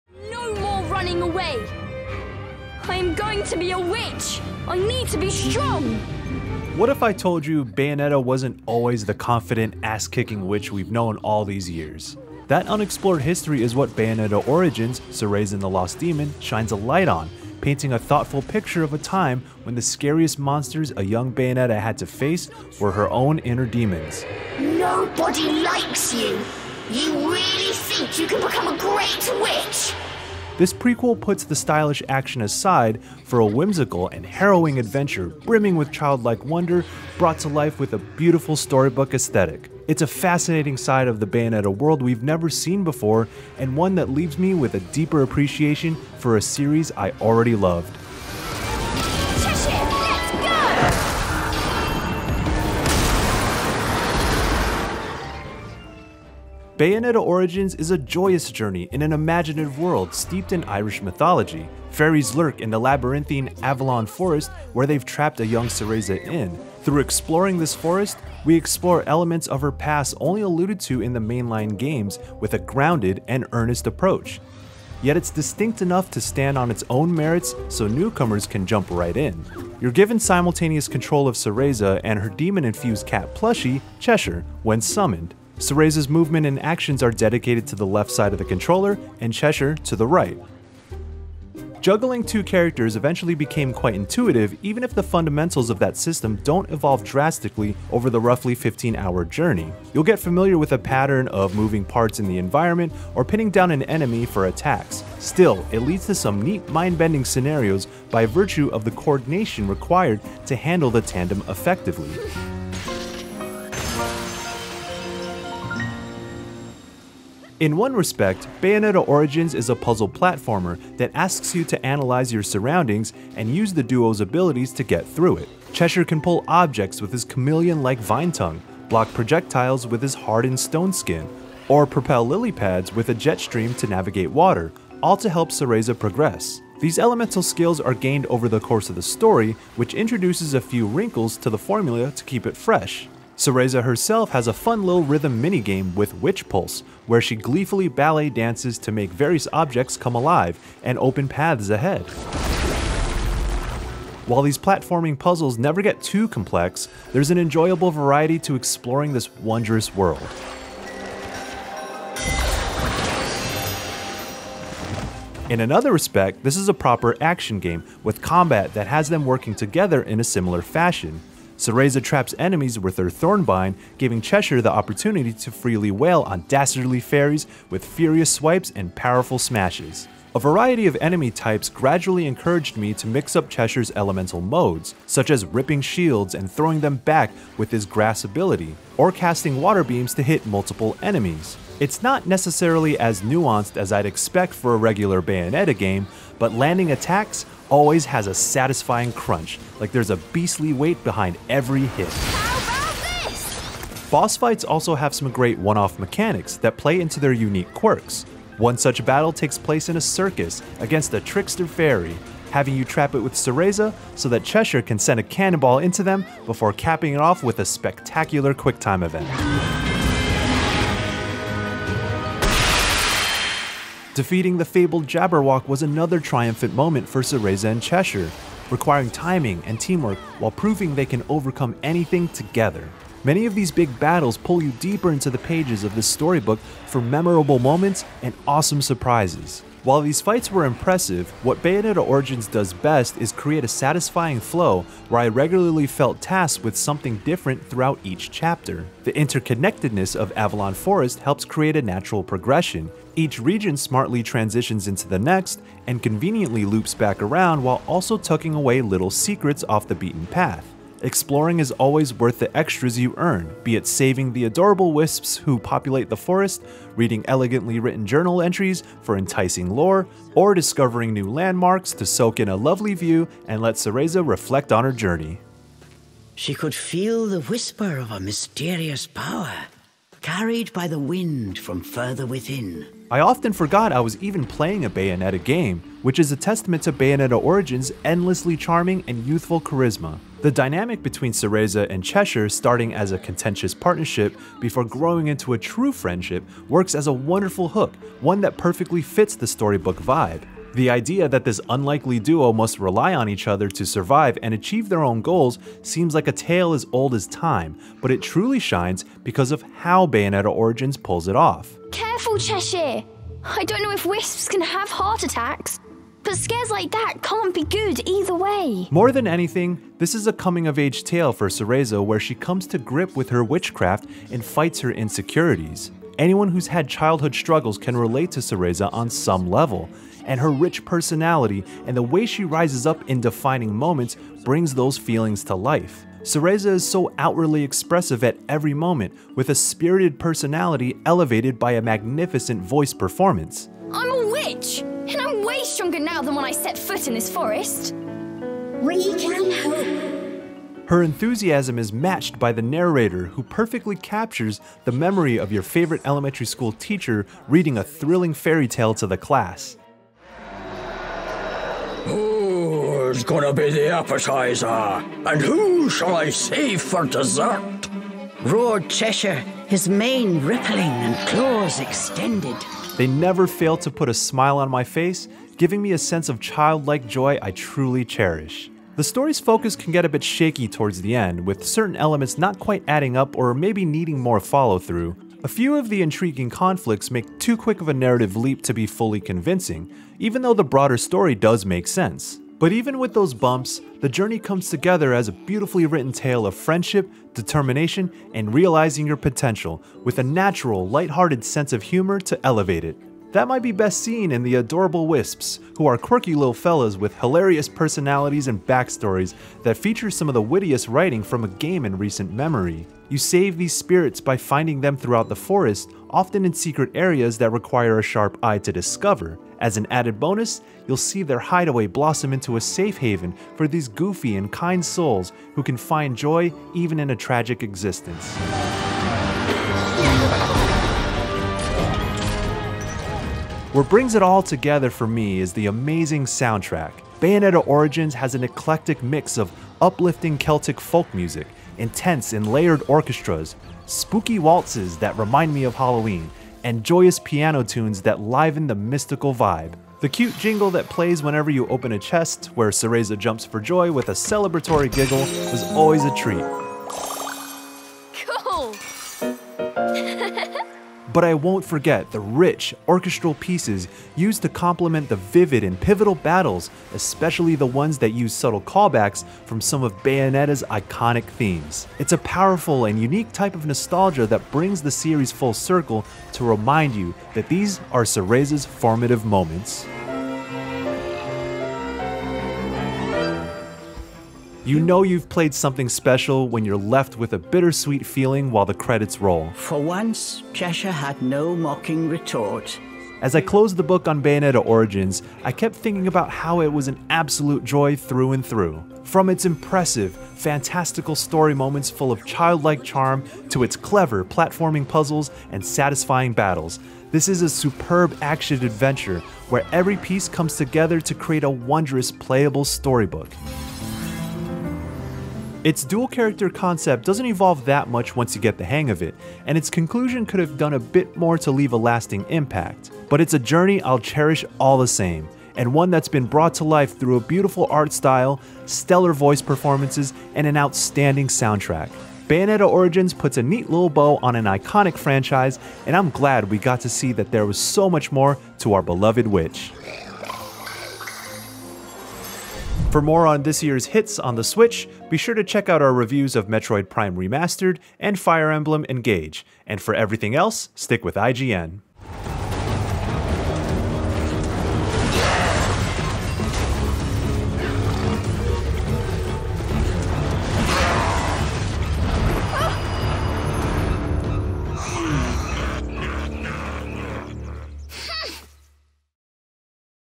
Bayonetta Origins: Cereza and the Lost Demon Review
Bayonetta Origins: Cereza and the Lost Demon reviewed by Michael Higham on Nintendo Switch.

This spin-off is an absolute joy through and through. From its impressive, fantastical story moments full of childlike charm to its clever platforming puzzles and satisfying battles, this is a superb action-adventure where every piece comes together to create a wondrous playable storybook. Its dual-character concept doesn’t evolve that much once you get the hang of it, and its conclusion could have done a bit more to leave a lasting impact. But it’s a journey you’ll cherish all the same, and one that’s been brought to life through a beautiful art style, stellar voice performances, and an outstanding soundtrack.